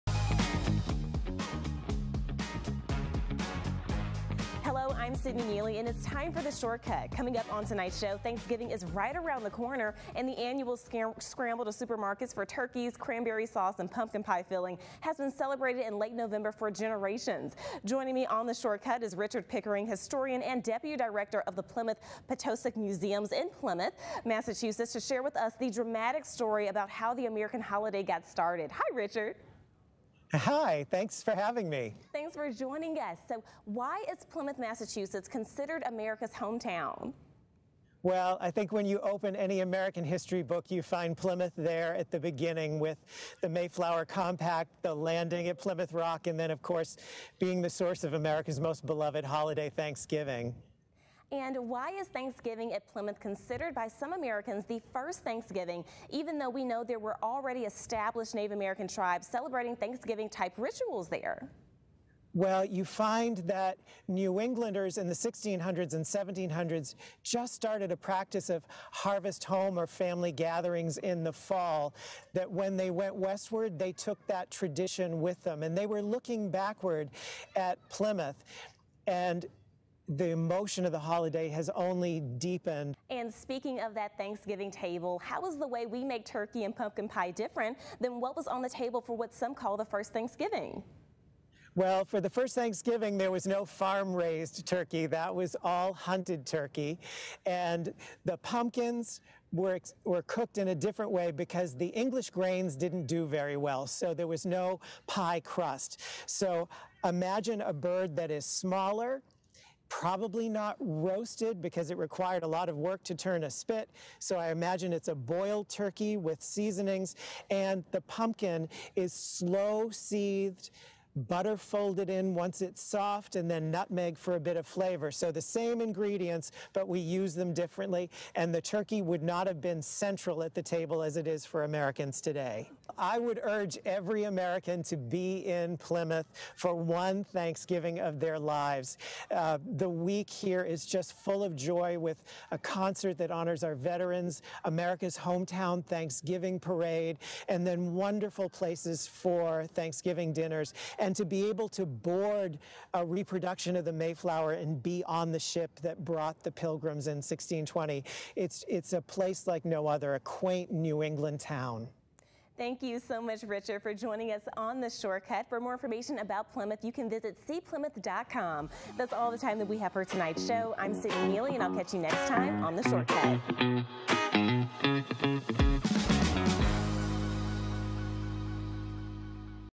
Discover the true story of Thanksgiving with Plymouth historian Richard Pickering!
Did you know that turkeys were not always the main star during Thanksgiving? Discover the true story and other traditions with Plymouth historian Richard Pickering!